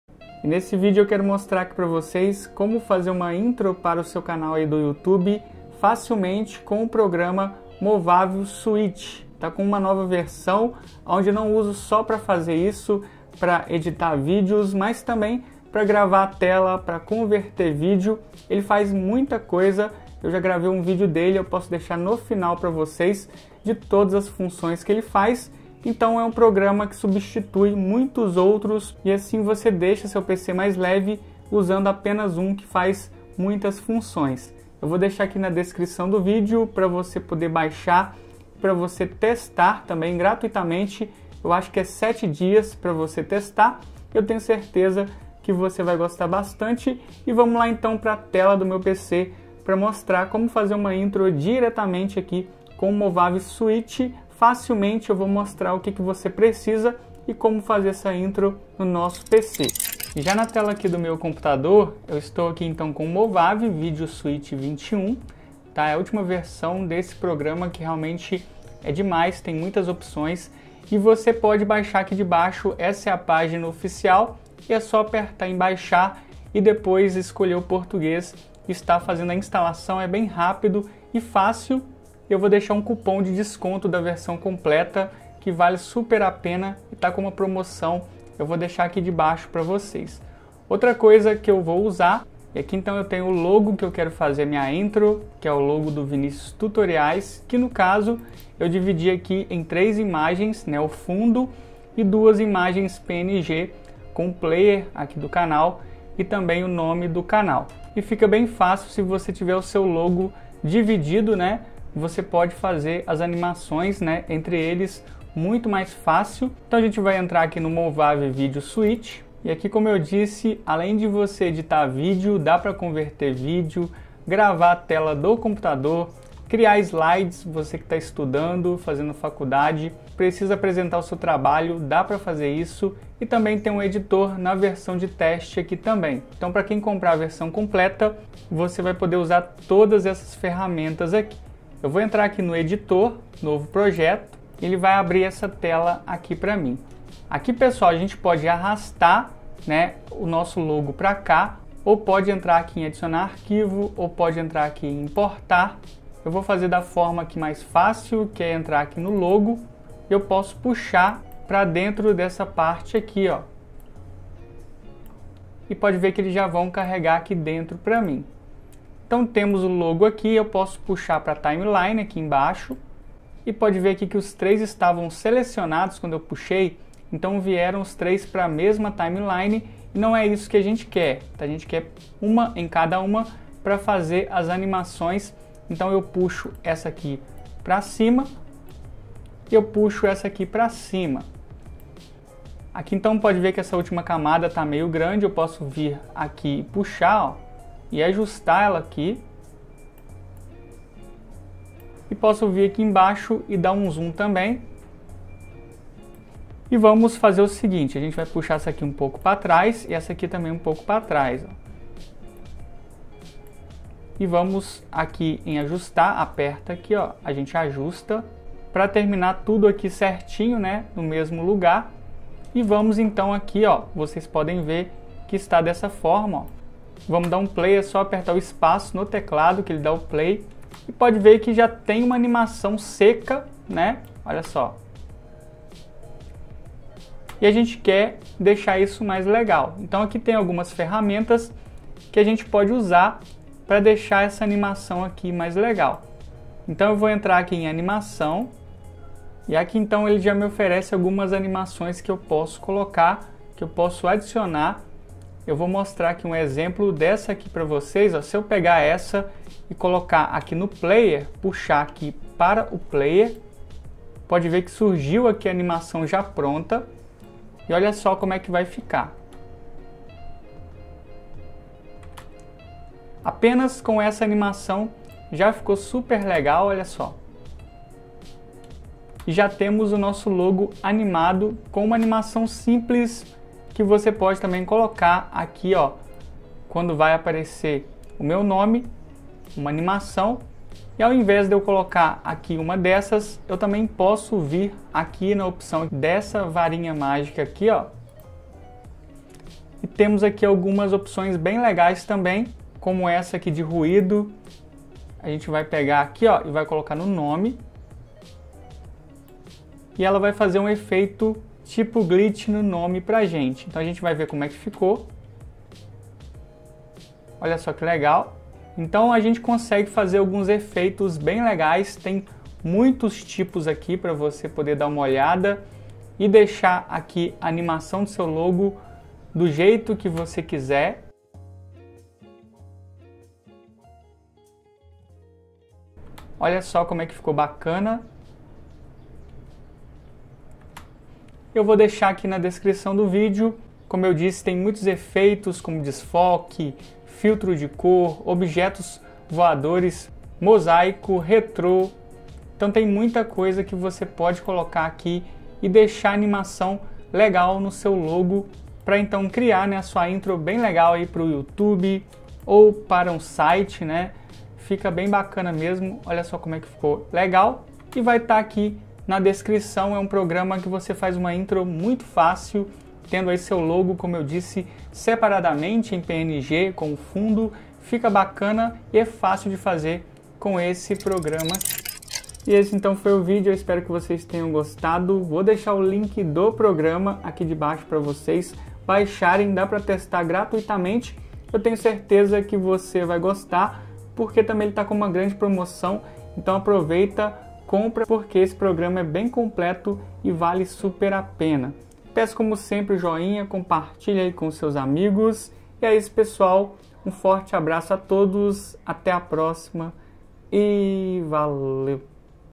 FORMA MAIS FÁCIL DE CRIAR UMA INTRO ANIMADA PARA SEU CANAL DO YOUTUBE
Vou estar ensinando como criar uma intro animada para seu canal do youtube mais facilmente e com o melhor programa.
Código promocional 20% - GIB21